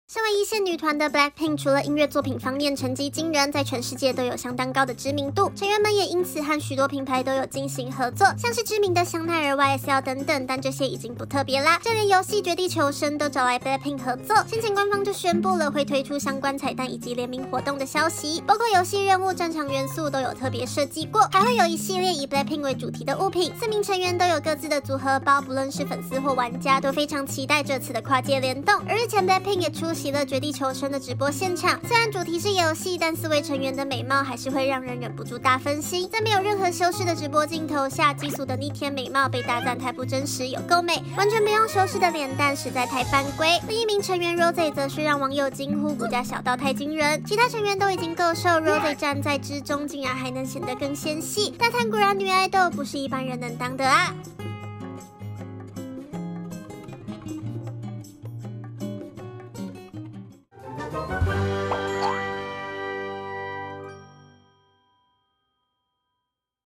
Jisoo展現零修圖逆天美貌 Rosé骨架小到站在成員旁也瘦一號
身為一線女團的#BLACKPINK
除了音樂作品方面成績驚人
在全世界都有相當高的知名度
成員們也因此和許多品牌都有進行合作
像是知名的香奈兒 YSL等等
但這些已經不特別啦
就連遊戲《絕地求生》
都找來BLACKPINK合作